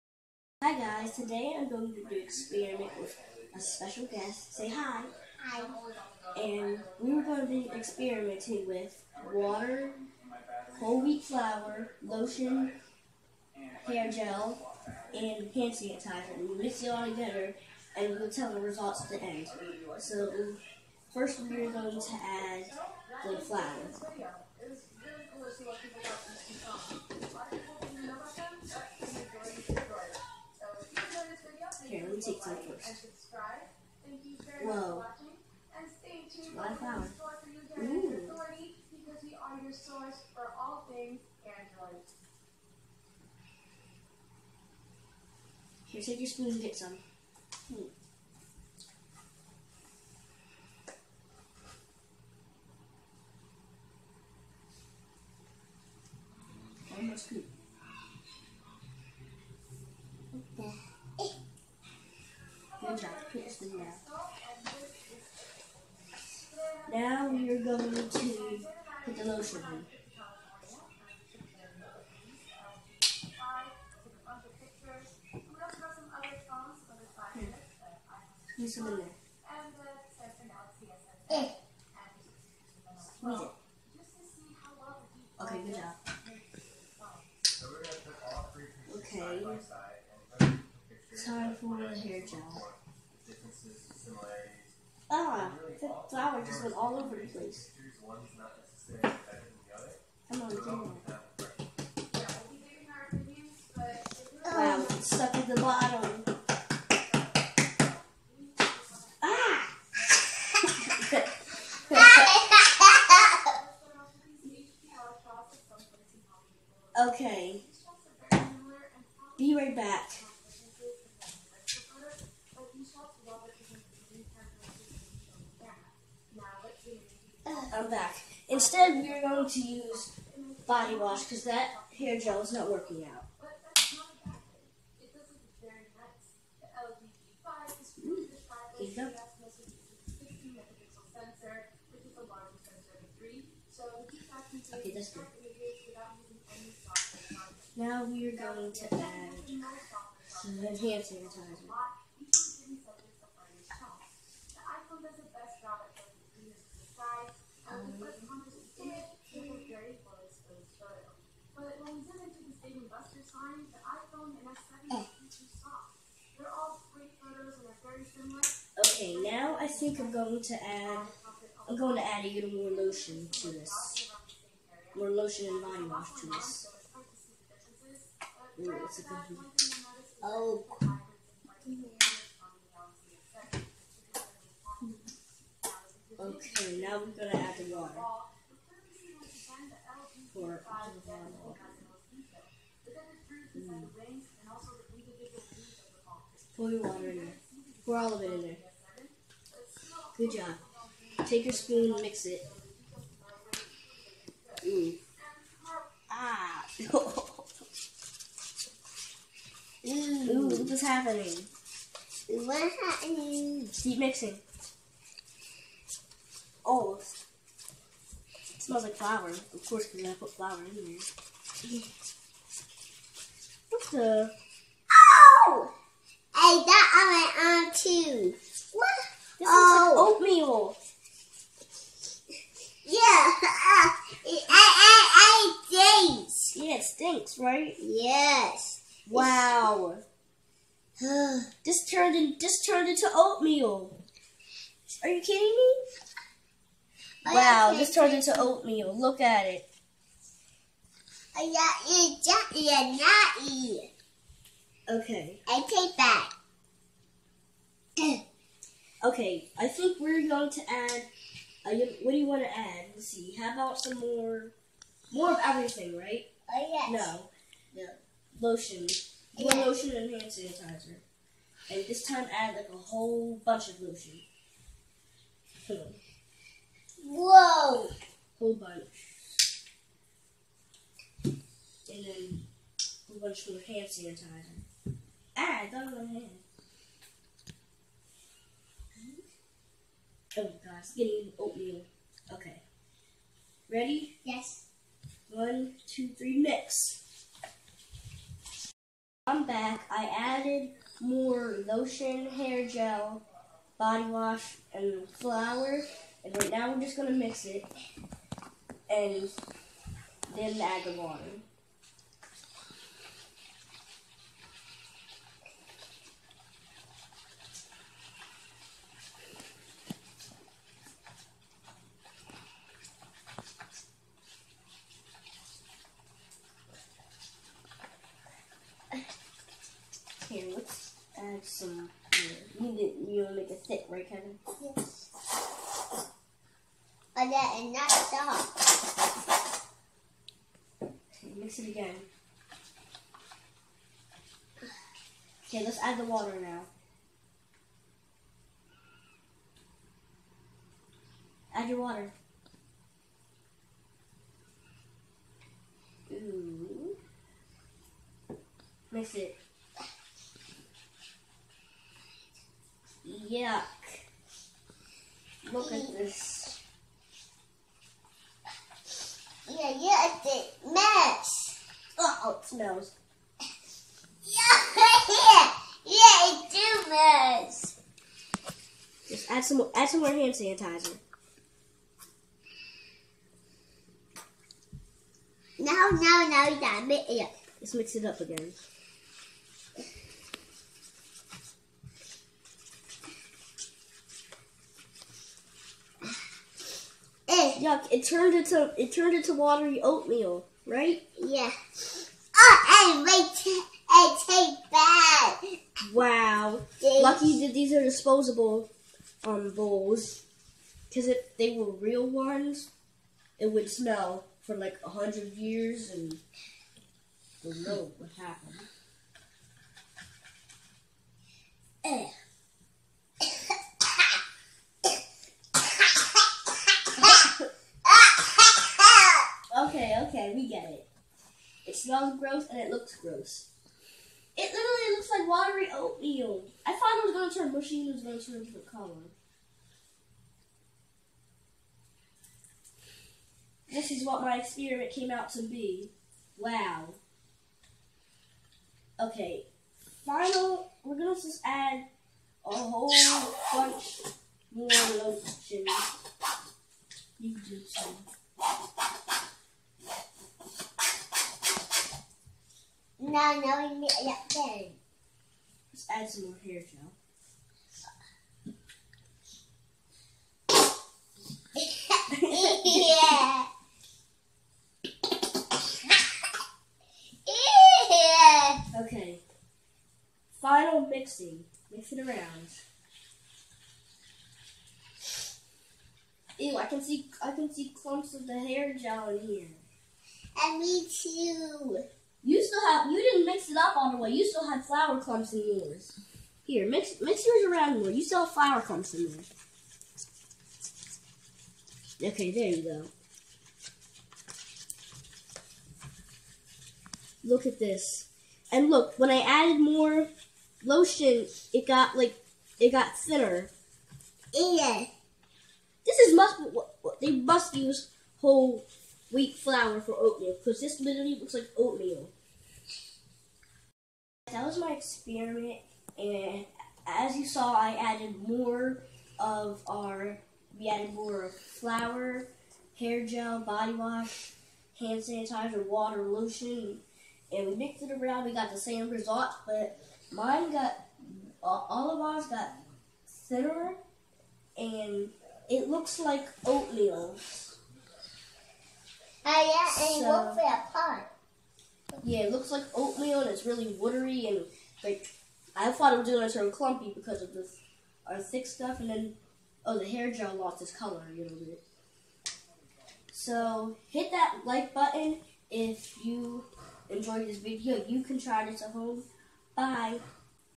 Experimenting With Whole Wheat Flower, Lotion, Hand Sanitizer, Body Wash, Hair Jel, and Water
Today me and a special guest will experiment with all these ingredients and mix them together and see what happens. You guys can do this at home too! Enjoy!